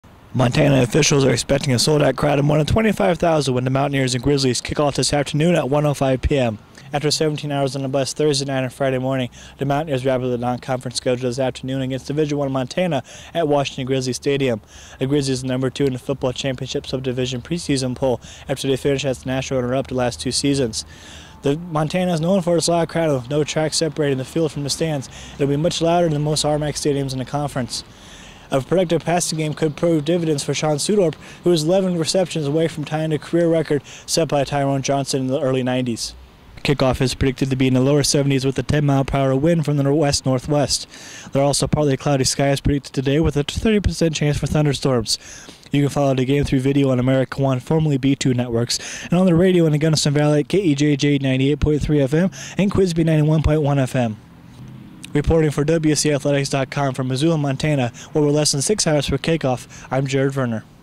Montana Gameday
Pregame preview for the Western State College Mountaineer football team against the Montana Grizzlies